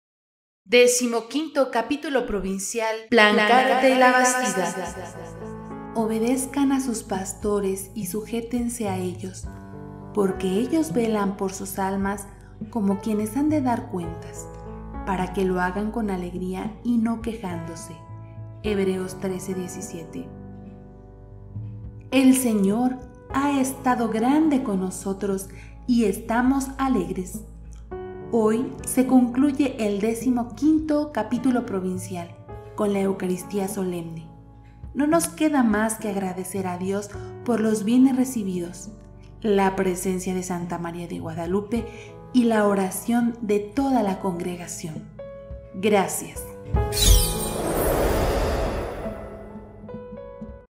XV Capítulo Provincial Plancarte y Labastida Dia 23 Oct.
Hoy concluye el XV Capítulo Provincial Plancarte y Labastida
Agradecemos a Dios todos los dones recibidos en este trabajo capitular.